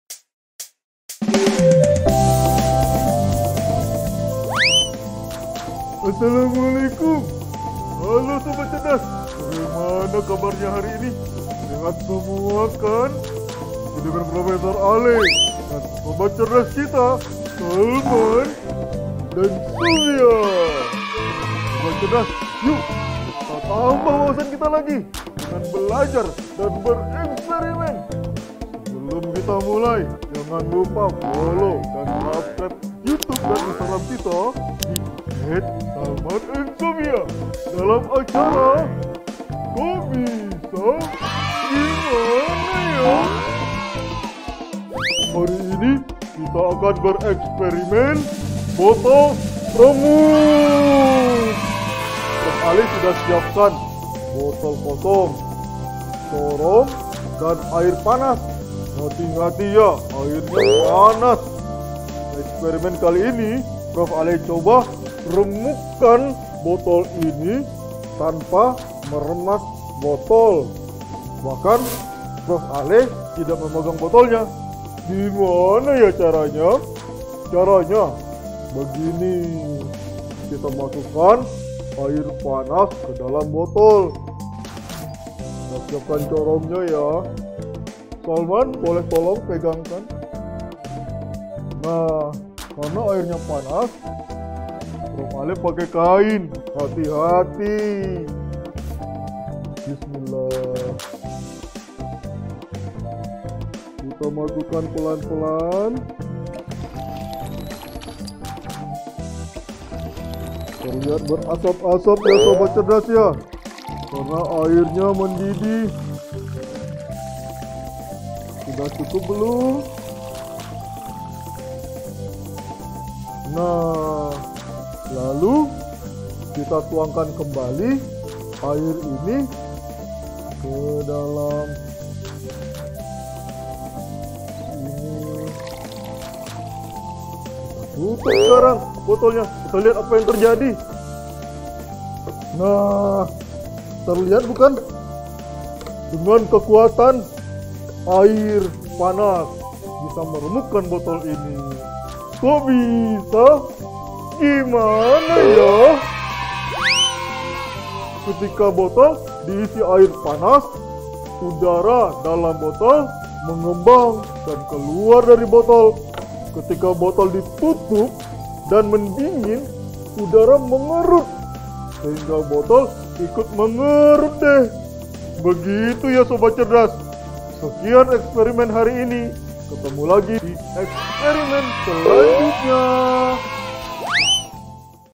Botol Plastik Remuk Tanpa Disentuh ?
Kali ini Prof Ale melakukan sedikit kekuatan gaib, bisa meremukkan botol bahkan tanpa menyentuh botolnya, hehe. Kok Bisa ya ?

Tapi ini ilmiah lo Sahabat Cerdas semua... Bukan sihir.